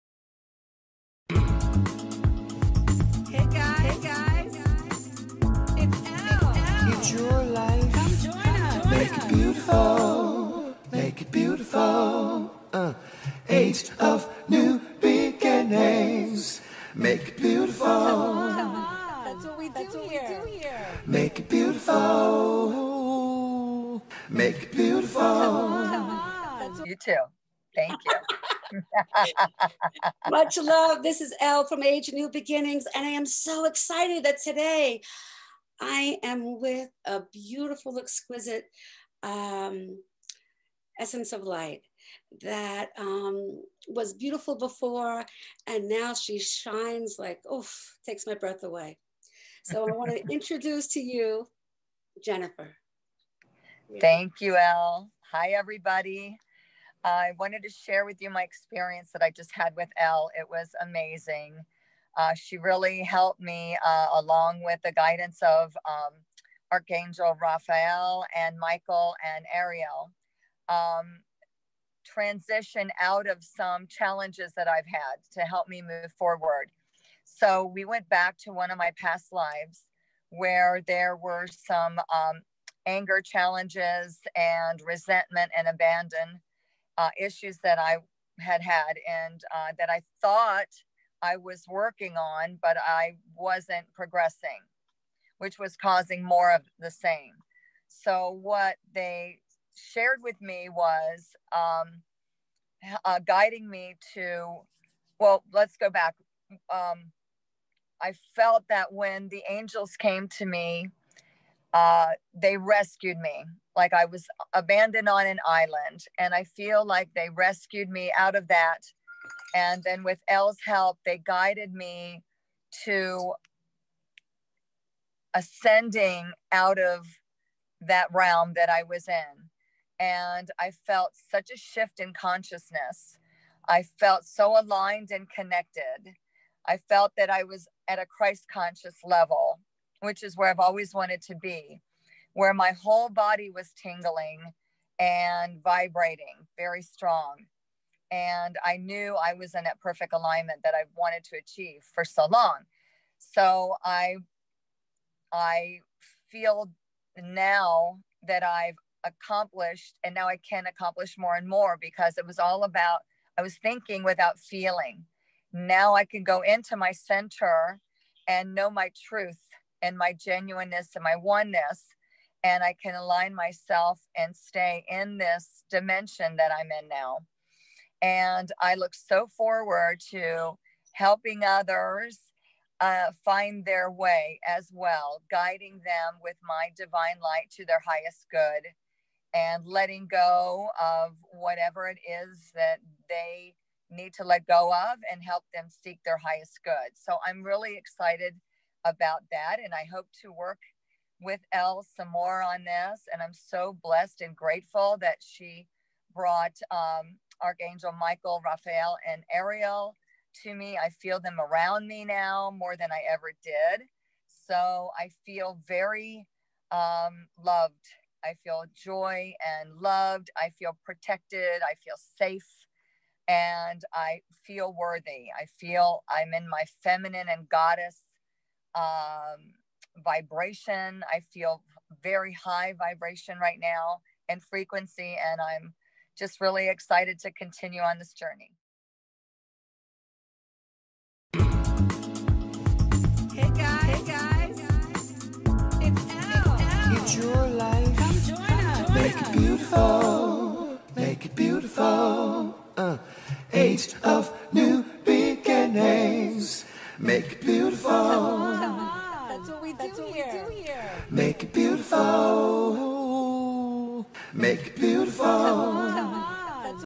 Age of New Beginnings;El's Testimonial of Jennifer's Healing Experience
This is Jennifer Judice's Quantum healing experience with Priestess El From Jan.23,2020